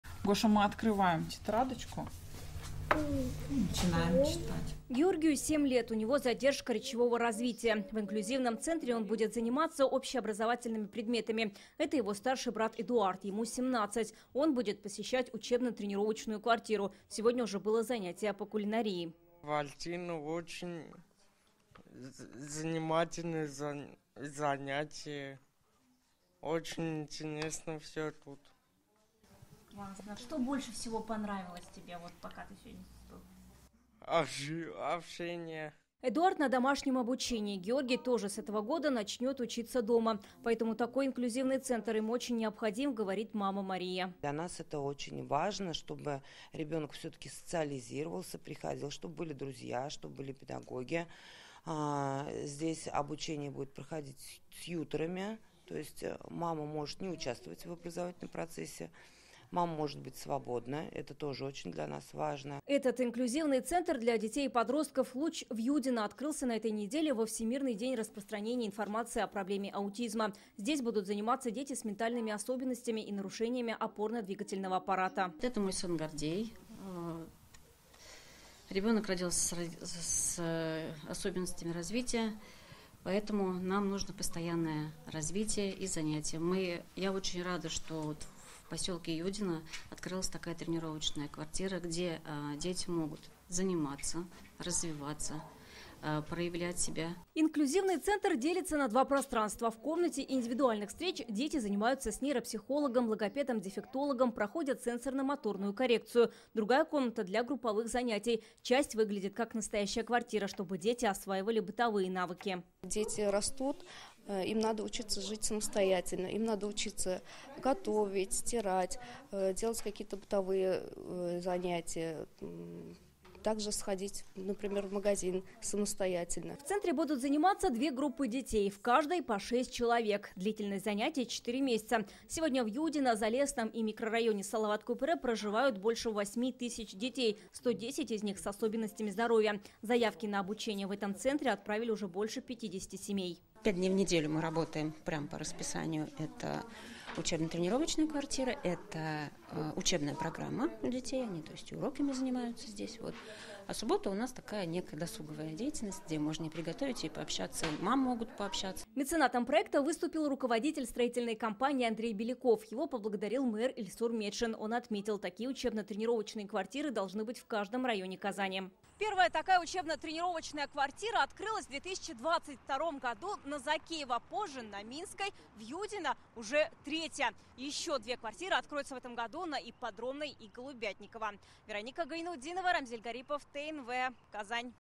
В Казанском районе Юдино открылся инклюзивный центр для детей и подростков с ментальными нарушениям
→ В Юдино открылся инклюзивный центр для детей и подростков "Луч". Это учебно-тренировочная квартира для ребят с ментальными нарушениями. Здесь они будут осваивать бытовые навыки, изучать школьную программу и посещать творческие занятия. Так дети с ограниченными возможностями смогут социализироваться и быть самостоятельными. На обучение в тренировочной квартире уже подали заявки больше 50 семей. Все они проживают в Юдино, Залесном и микрорайоне Салават Купере. Это уже третий подобный инклюзивный центр в Казани. Подробнее в репортаже Вероники Гайнутдиновой.

Координаты:
☎ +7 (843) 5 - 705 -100